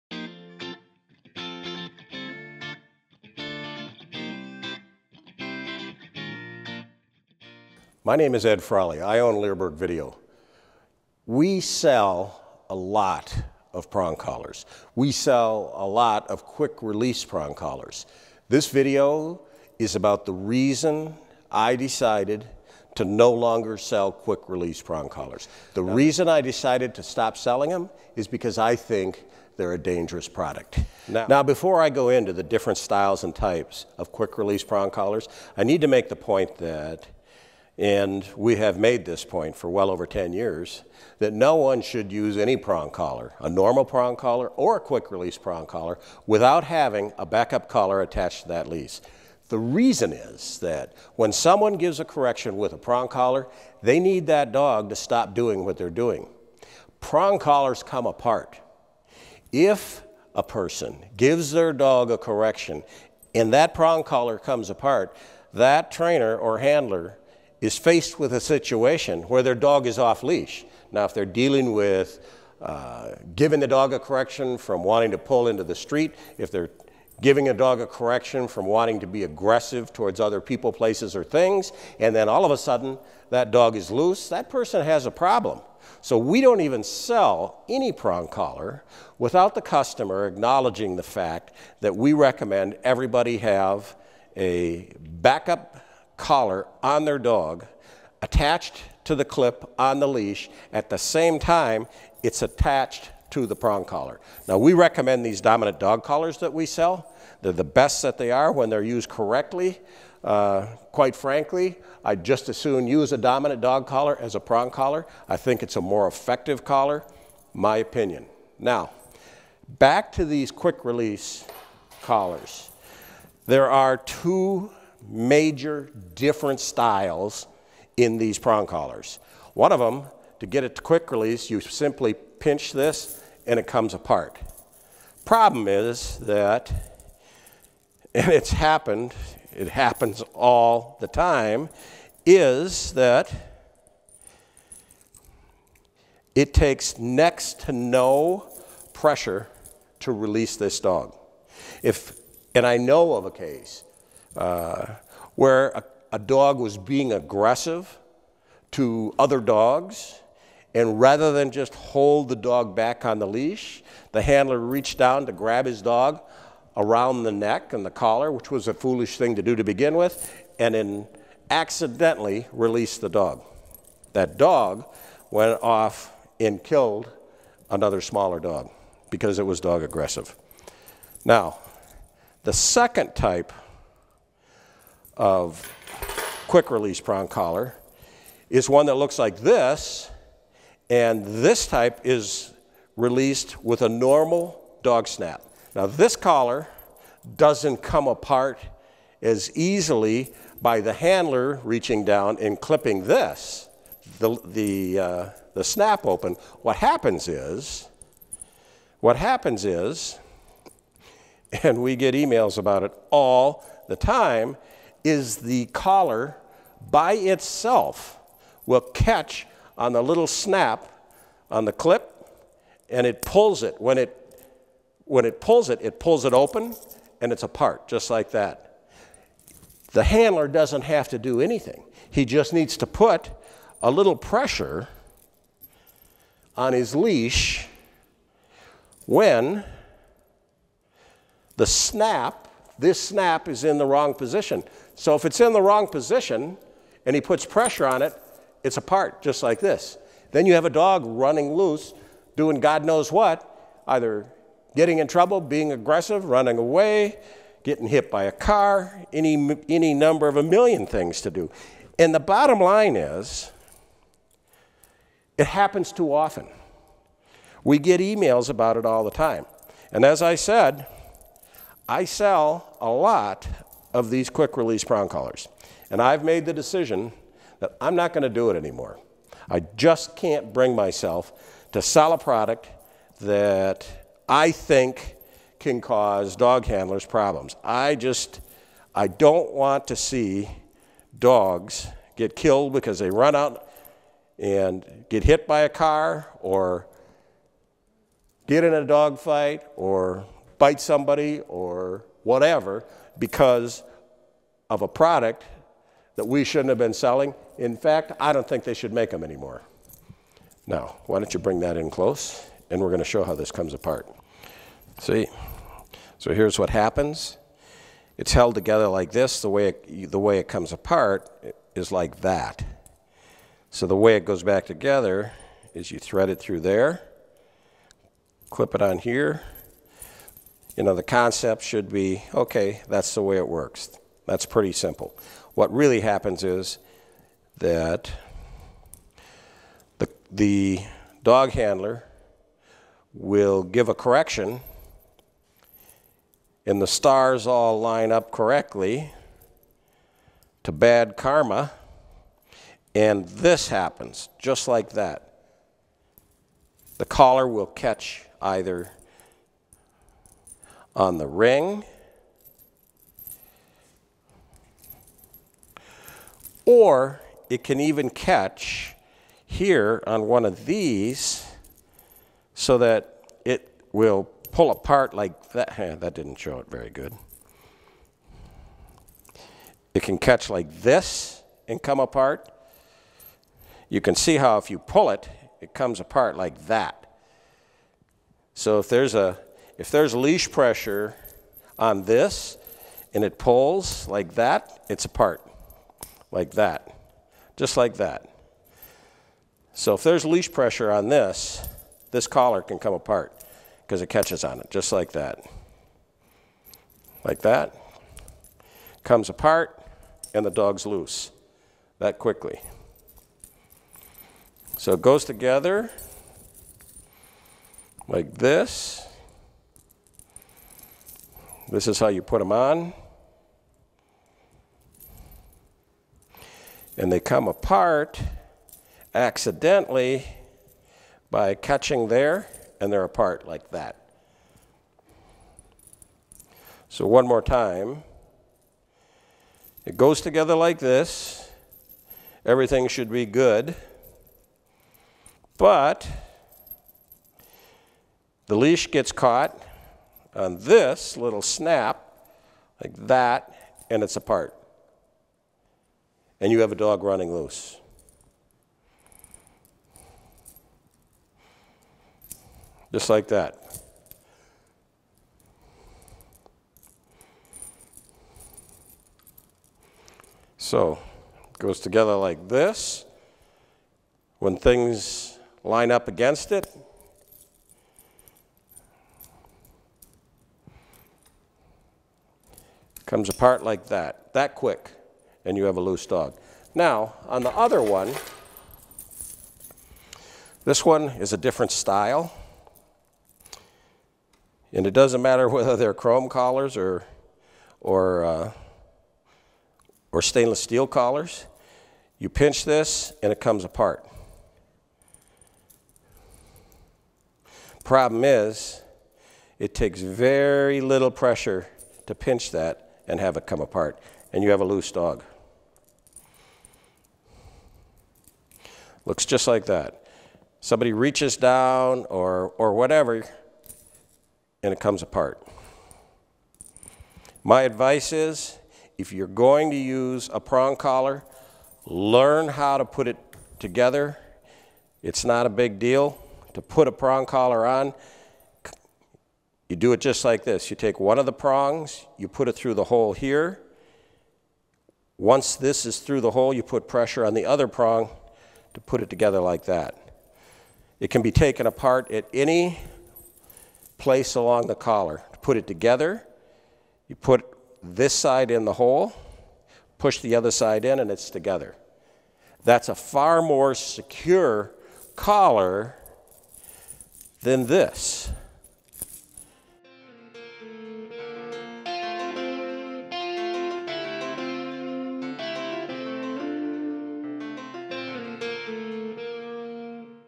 Why Leerburg Doesn’t Sell Quick Release Prong Collars
In this video, Ed Frawley explains why he will no longer sell quick-release prong collars like the styles shown in the video. The quick-release collars shown in this video are not to same as our easy snap prong collars that we sell on our website.

Leerburg strongly recommends the use of a dominant dog collar as a safety backup collar when using a prong collar. Prong collars can come apart or break at any time. With the use of a prong collar leash or prong collar adapter, you can clip the leash to both the prong collar and the backup collar and be able to retain control in the rare occurrence that the prong collar fails.

To learn how to correctly fit a prong collar, go or purchase our Basic Dog Obedience DVD/Stream/LOU